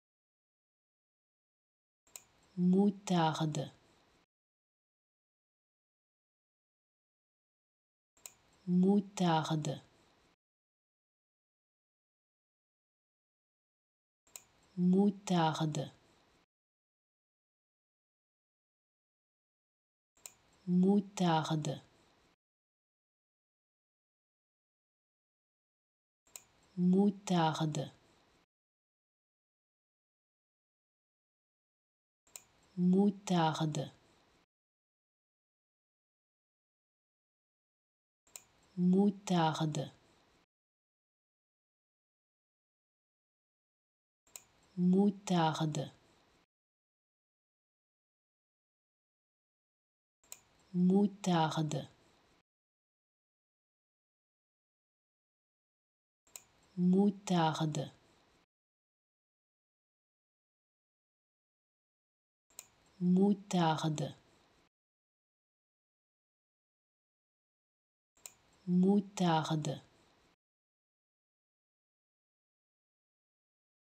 How to Pronounce ''Moutarde'' (Mustard) Correctly in French
Learn how to say and properly pronounce ''Moutarde'' in French with this free pronunciation tutorial. Apprenez à prononcer des mots en français grâce à nos tutoriels.